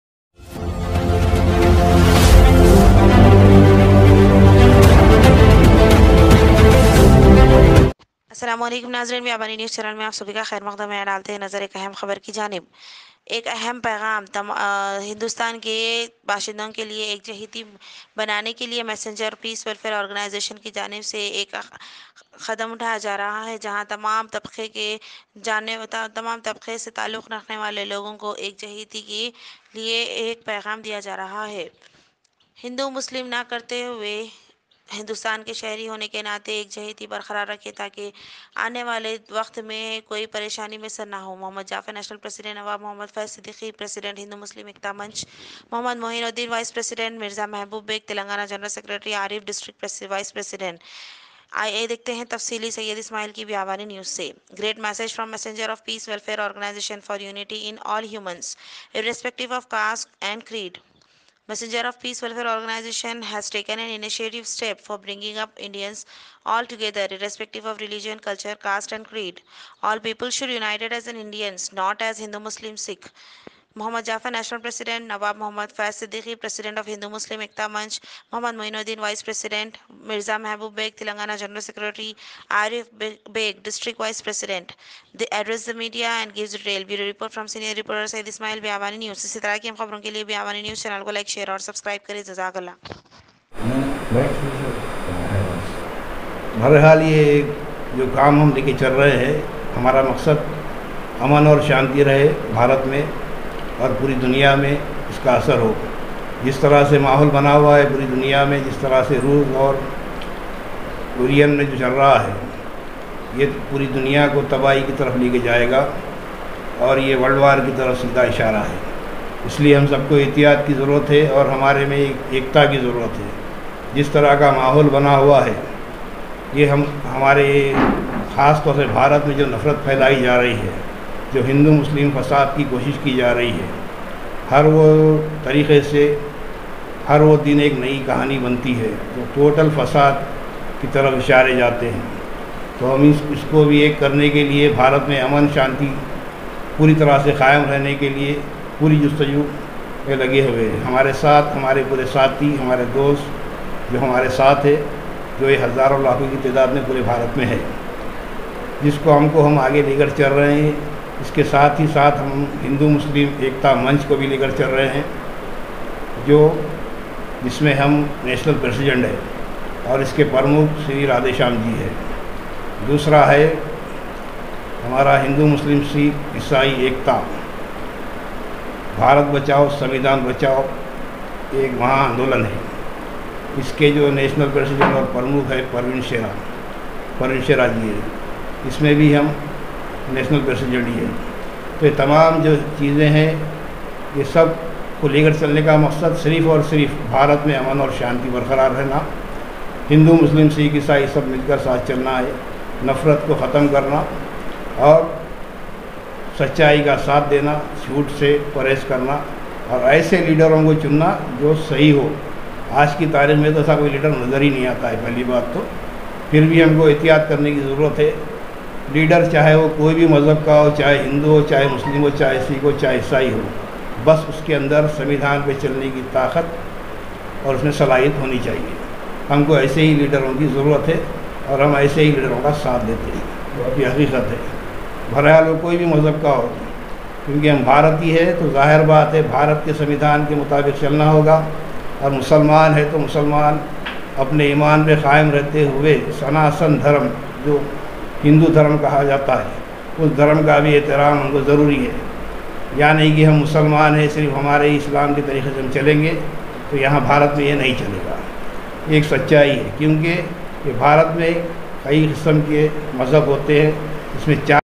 GREAT MESSAGE FROM MESSENGER OF PEACE WELFARE ORGANIZATION FOR UNITY IN ALL HUMANS Irrespective of
GREAT MESSAGE FROM MESSENGER OF PEACE WELFARE ORGANIZATION FOR UNITY IN ALL HUMANS Irrespective of Caste and Creed Biabani News
 Messenger of Peace welfare organization has taken an initiative step for bringing up Indians altogether irrespective of Religion, Culture, Caste and Creed, all people should united as an Indians not as Hindu,Muslim,Sikh ..
MOHD JAFFER National President,
Nawab Mohammed Faiz Siddiqui president of Hindu Muslim ekta munch,
Mohd Moinuddin Vice-president,
Mirza Mehboob Baig Telangana General Secretary,